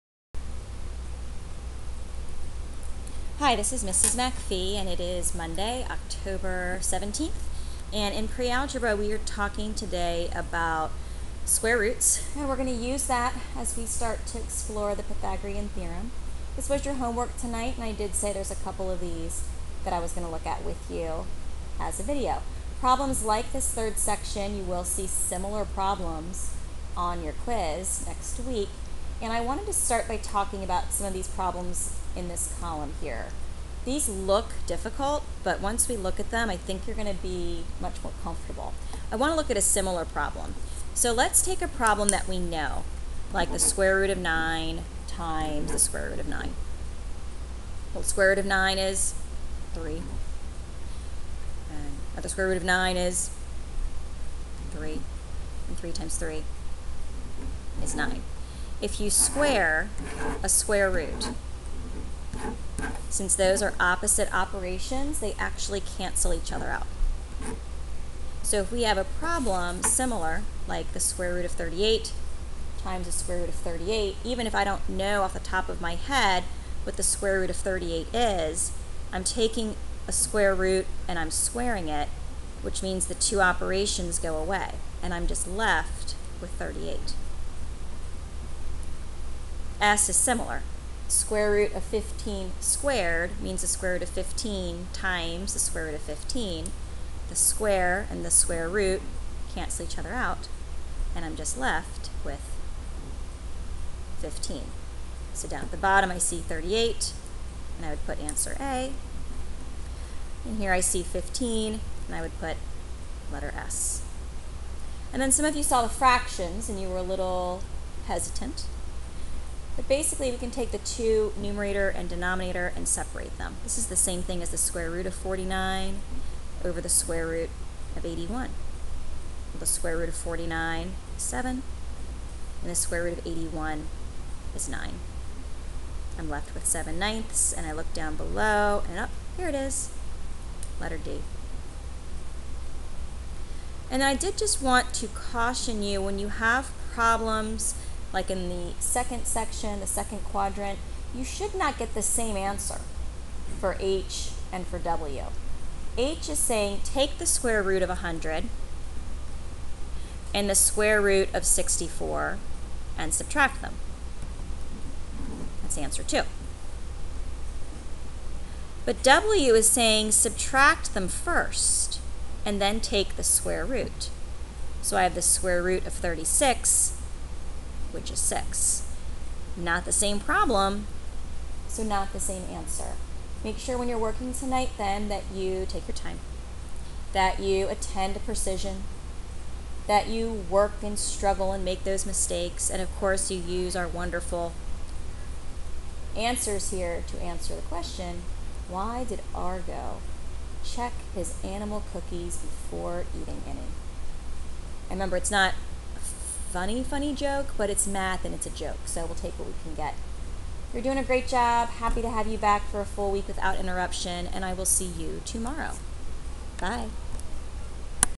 Operations with Squares and Square roots
Mrs. MacPhee, a middle school math teacher, offers tips and examples to further aid and enrich her students.